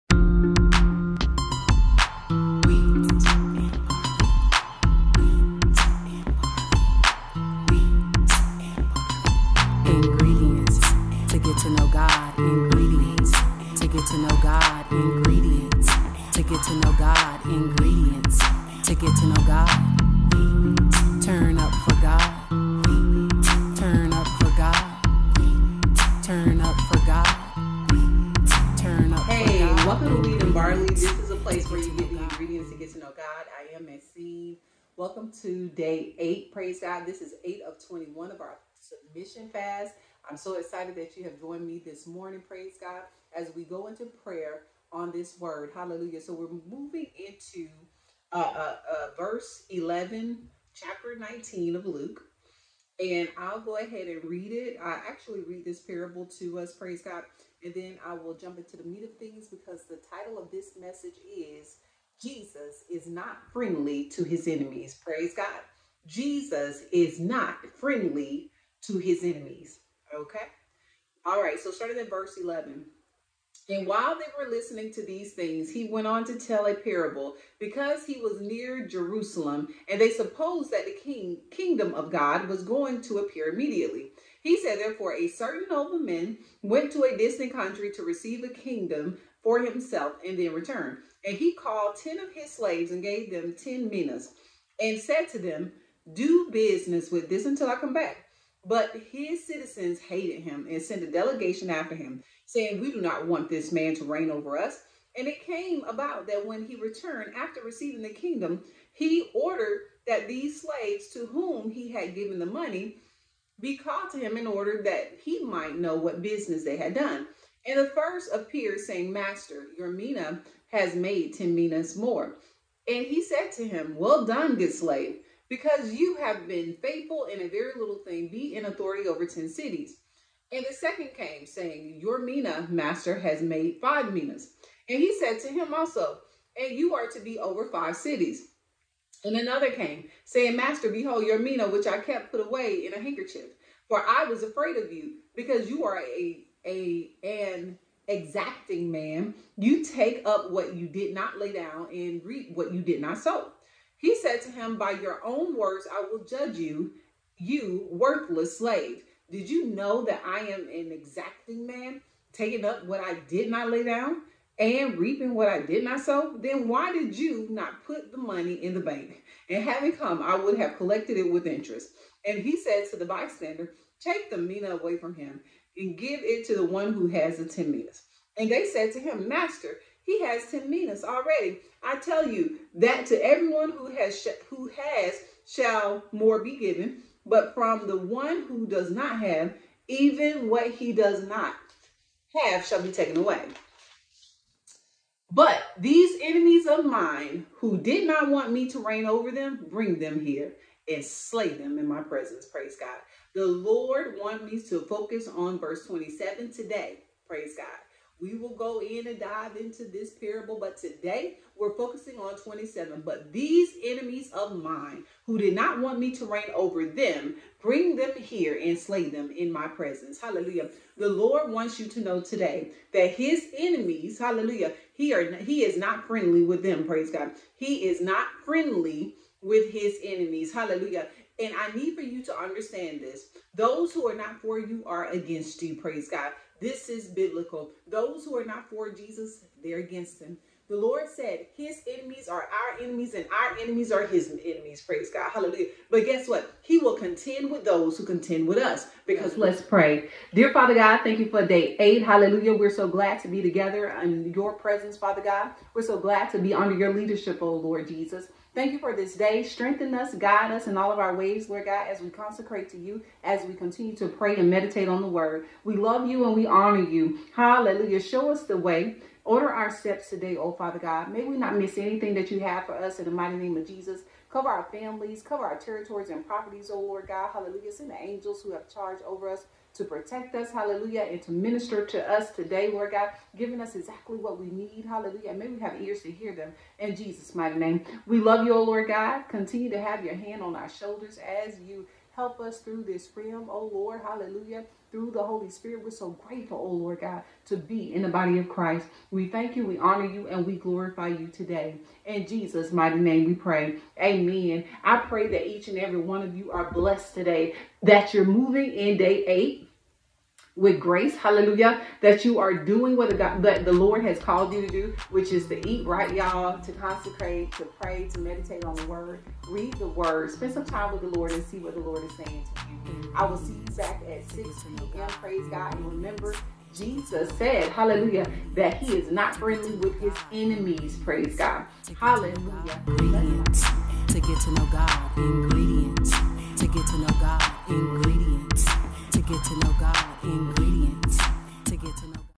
Submission Fast Day 8 of 21: 6AM EST Morning Prayer| Jesus is Not Friendly With His Enemies
This message is sent forth by the Lord! All Glory to God!